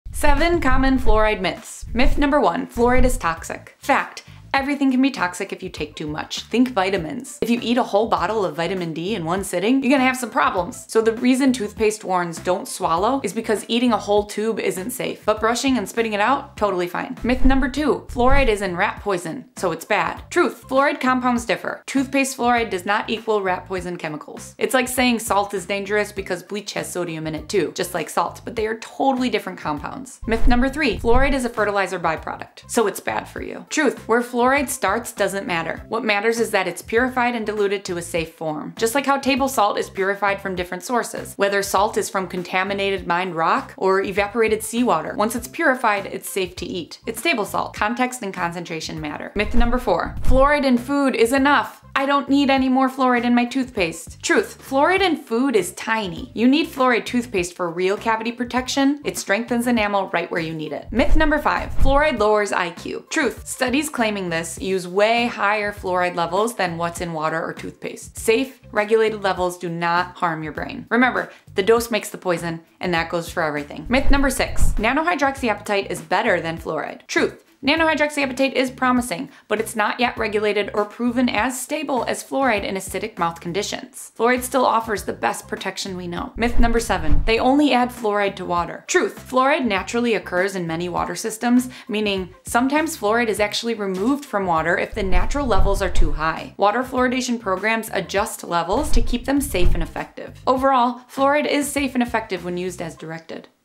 fluoride myths debunked
7 Fluoride Myths—Busted!

Let’s clear the (toothpaste) air! From “toxic” fears to the nano-hydroxyapatite debate, fluoride myths are *everywhere*. But science says otherwise. 👩‍🔬✨

✅ Fluoride is safe.
✅ Fluoride is effective.
✅ Fluoride helps prevent cavities.

Let’s stop the misinformation and protect our smiles. Which myth have *you* heard the most?

Remember to focus on fluoride FACTS, not fear.